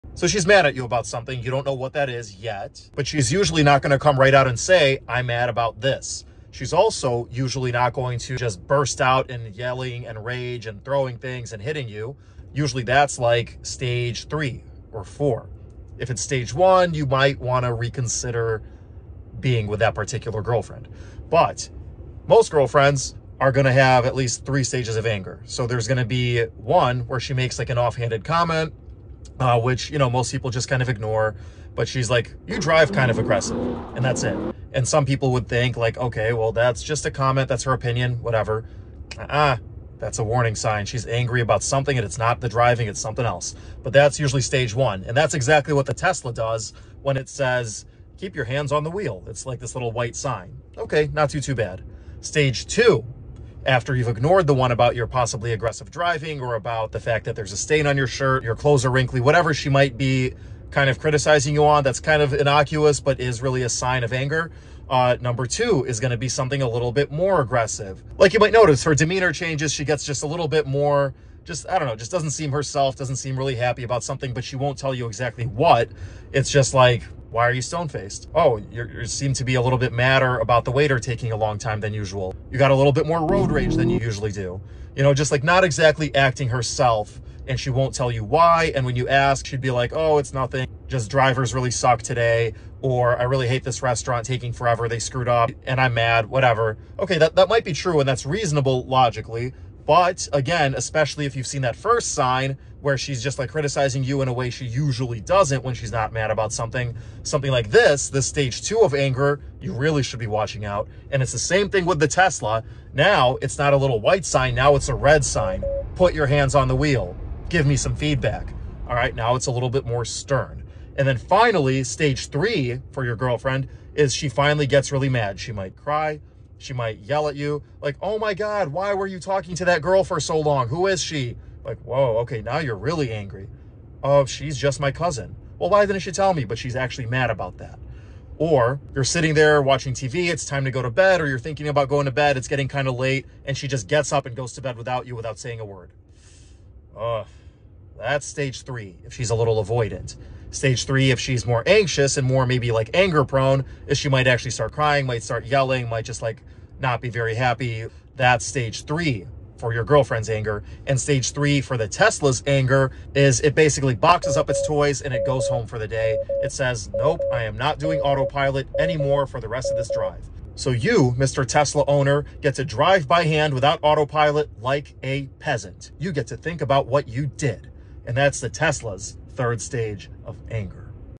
Teslas and Girlfriends
Just my 2¢

I can't help but see some parallels between how my new (used) car, Gigi, a 2016 Tesla Model S 75D, kicks me out of autopilot when I don't pay attention to her, and how your girlfriend gives you at least two subtle warning signs that she's mad about something before most guys realize she's mad. Enjoy!